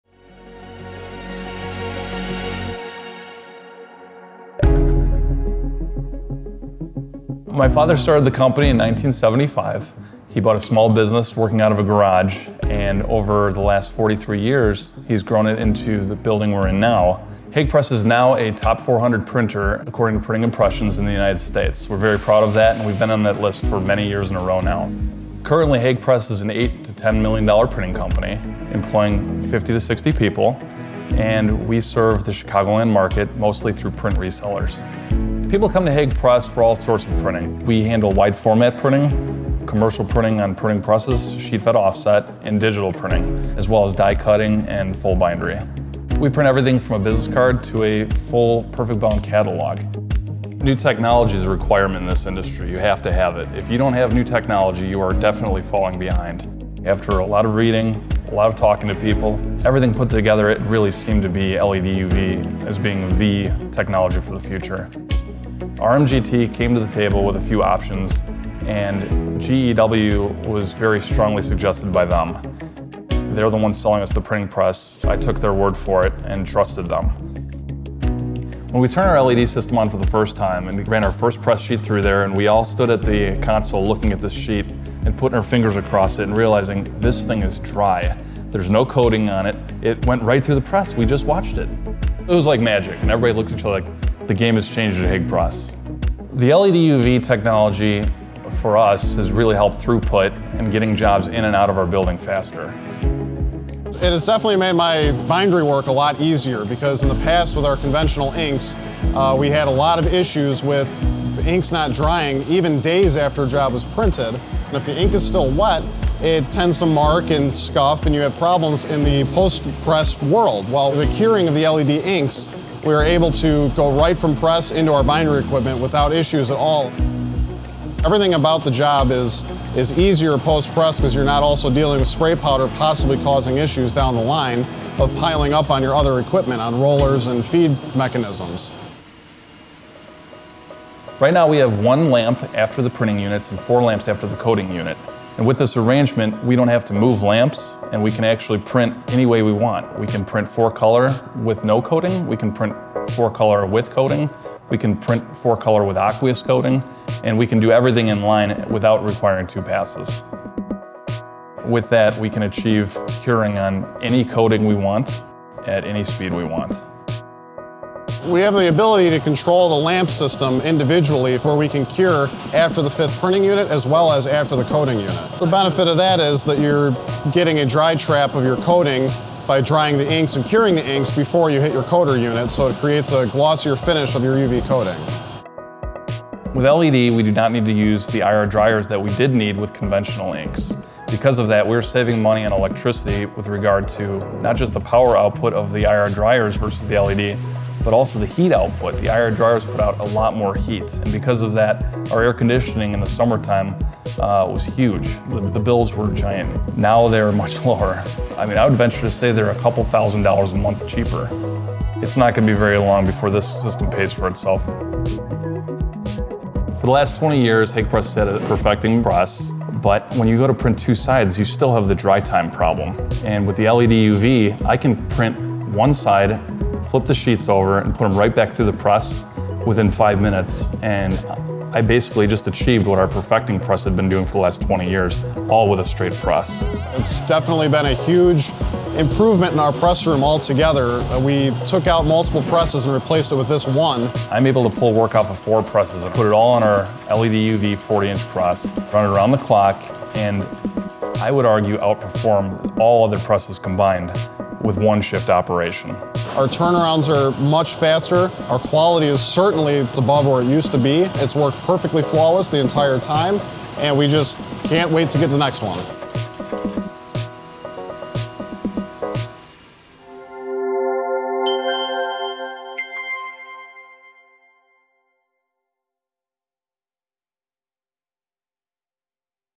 GEW Customer Spotlight Hagg Press
Hagg Press is a ‘Top 400 Printer’ according to Printing Impressions in the USA. The family company has been on this prestigious list for many years now. Established in 1975, Hagg Press is based in Elgin, Illinois and serves the thriving Chicagoland print market. Business is largely through resellers and is extremely competitive.

The management team at Hagg are strong believers in new technology. In the words of Jordan Hagg, Vice President of the company: “New technology is a requirement in this industry - you have to have it - if you don't have it, you are definitely falling behind.” This pioneering approach led to their first installation of a GEW LED UV curing system in 2018, retrofitted to their RMGT 10-Series press.

The decision to install LED was largely driven by the press manufacturer, as Jordan explains: “After a lot of reading, a lot of talking to people, everything put together it really seemed to be LED UV as being the technology for the future. RMGT came to the table with a few options and GEW was very strongly suggested by them.” He goes on: “When we turned our LED system on for the first time and ran our first press sheet through there and we all stood at the console looking at this sheet and putting our fingers across it and realizing 'this thing is dry... there's no coating on it, it went right through the press, we just watched it.' It was like magic and everybody looked at each other like... the game has changed at Hagg Press.”

Brother to Jordan, Taylor Hagg is also a company VP. He focuses most of his time on production output, which has been transformed by the installation of LED UV. He explains: “The LED UV has definitely made my bindery work a lot easier because in the past with our conventional inks, we had a lot of issues with the inks not drying even days after a job was printed... and if the ink is still wet it tends to mark and scuff and you have problems in the post-press world. While with the curing of the LED inks, we are able to go right from press into our bindery equipment without issues at all. Everything about the job is easier post-press because you're not also dealing with spray powder possibly causing issues down the line, of piling up on your other equipment, on rollers and feed mechanisms.”

The press is installed with a GEW LeoLED Modular Lamp Array (MLA) system, which gives the print team a great deal of production flexibility, as Jordan Hagg clarifies: “Right now we have one lamp after the printing units and four lamps after the coating unit. With this arrangement we don't have to move lamps and we can actually print any way we want - we can print four colour with no coating, we can print four colour with coating, we can print four colour with aqueous coating - and we can do everything in line without requiring two passes. With that we can achieve curing on any coating we want, at any speed we want.”

They also have the ability to control the lamp system individually, where they can cure after the fifth printing unit as well as after the coating unit. This means that they can achieve a dry trap of the coating by curing the inks before they reach the coater unit, which creates a glossier finish to the UV coating.

With the installation of the LED UV curing system, Hagg can now do the work of several presses with just the one RMGT 10 press. In the words of Jordan: “For the last 20 years Hagg Press had a perfecting press but when you go to print two sides you still have the dry time problem and with the LED UV I can print one side, flip the sheets over and put them right back through the press within five minutes - and I basically just achieved what our perfecting press had been doing for the last 20 years – all with a straight press. I'm able to pull work off of four presses and put it all on our LED UV 40-inch press, run it around the clock and I would argue outperform all other presses combined with one shift operation.”

On the question of the return on investment period for the LED UV system, Jordan comments: “With LED we do not need to use the IR dryers that we did need with conventional inks... because of that we're saving money on electricity, with regard to not just the power output of the IR dryers versus the LED, but also the heat output - the IR dryers put out a lot more heat and because of that our air-conditioning in the summertime was huge - the bills were giant - now they're much lower. I would venture to say they are a couple of thousand dollars a month cheaper. It's not going to be very long before this system pays for itself.”

Taylor Hagg concludes: “With LED our turnarounds are much faster, our quality is certainly above where it used to be, it's worked perfectly flawless the entire time and we just can't wait to get the next one.”

For information on GEW LED UV for sheetfed printing, go

Have any questions?